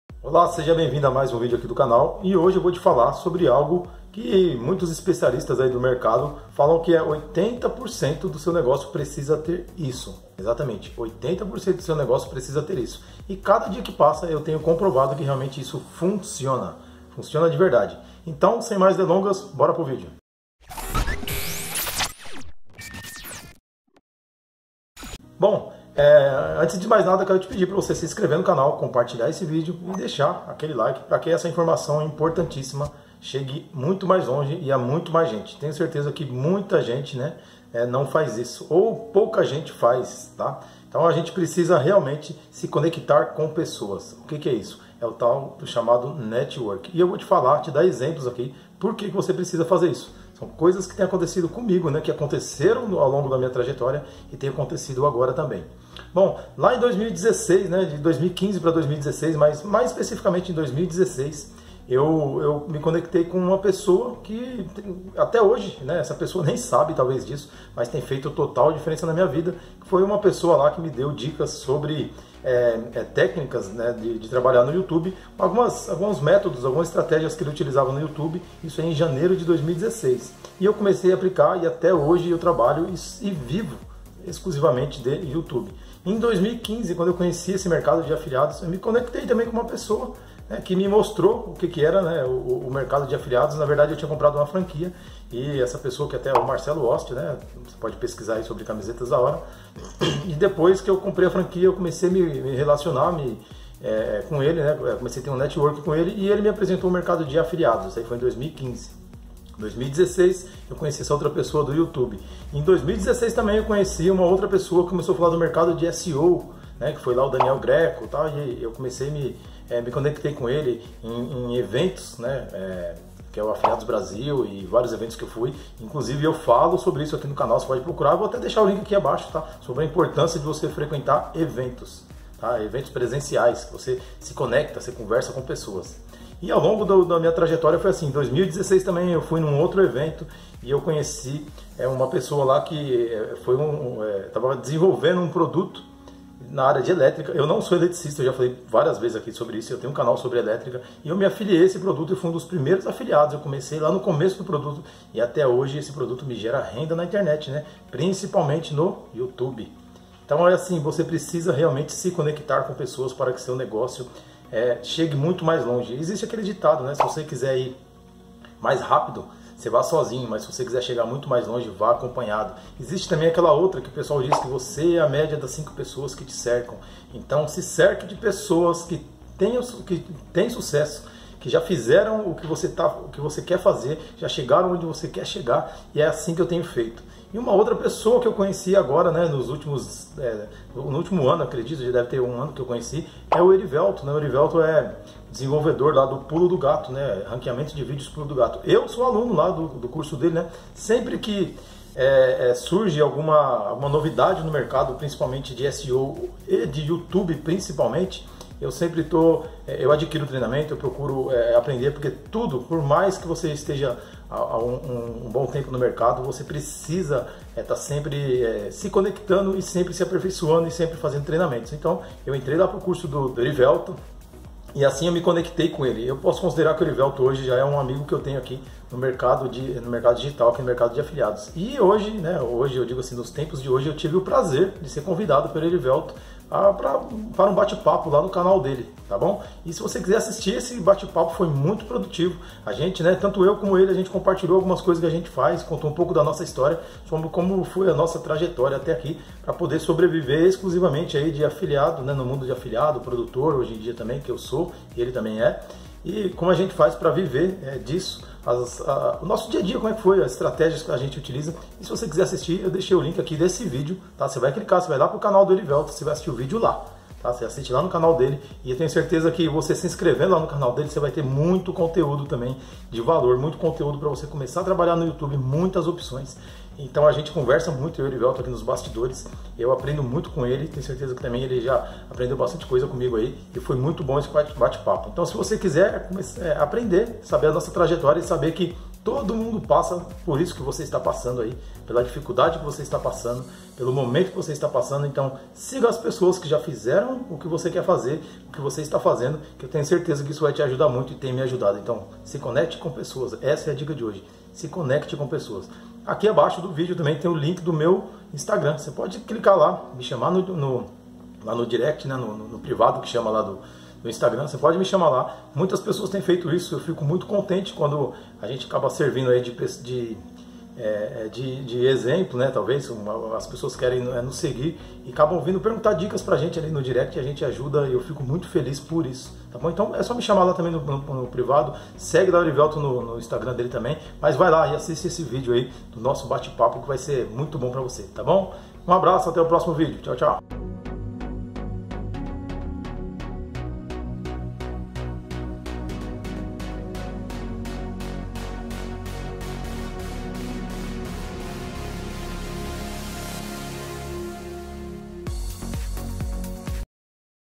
Bate Papo com Erivelton Ranqueamento de Vídeos Pulo do Gato | NETWORKING VALE MAIS QUE DINHEIRO
CONEXÃO E NETWORKING Bate Papo com Erivelton Ranqueamento de Vídeos Pulo do Gato

Fazer boas conexões e Networking é essencial para o nosso crescimento pessoal e profissional.

Alguns dizem que Networking vale mais que Dinheiro e que isso é responsável por 80% dos seus resultados.

Tive a Honra de ser convidado pelo Erivelton, do treinamento ranqueamento pulo do gato, para um bate papo em seu canal do Youtube.
Acompanhe que será muito útil para seu crescimento profissional.